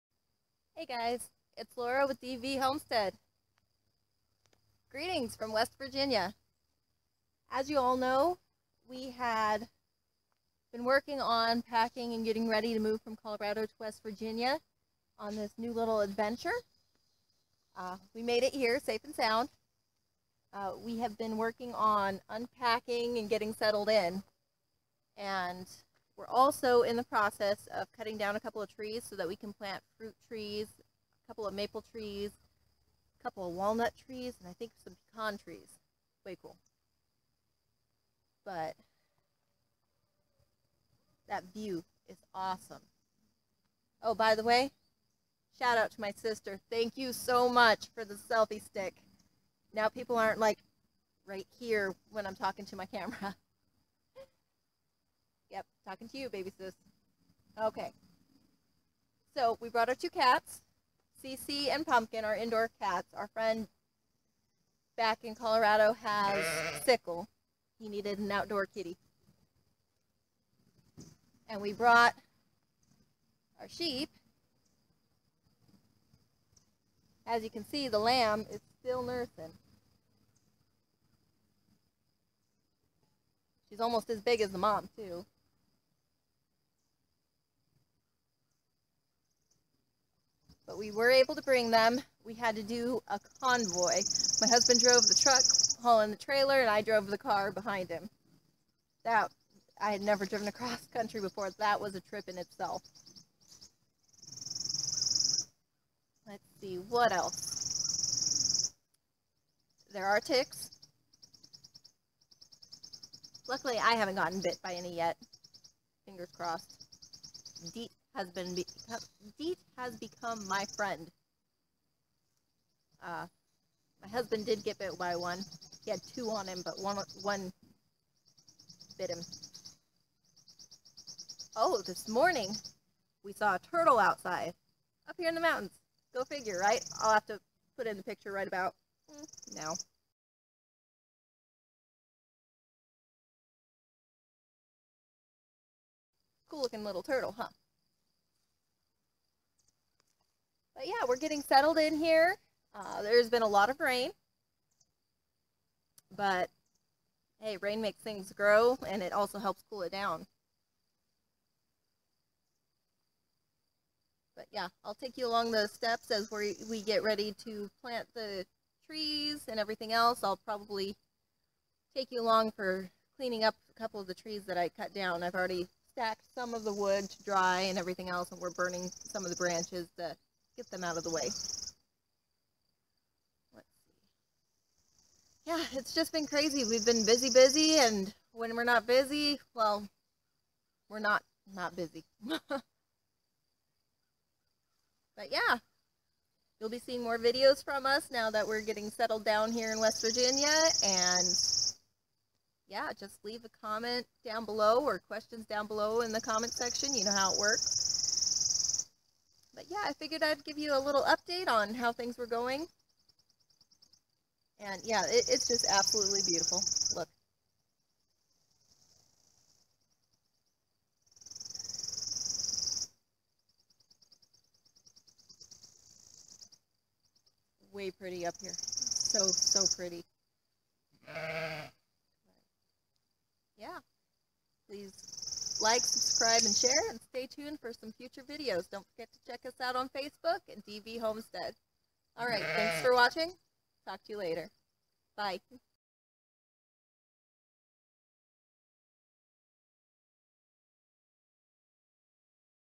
Greetings from West Virginia!
We made it to West Virginia! Our homesteading adventure continues! Here is a little update with upcoming projects!
Thsnks for watching!
-Lora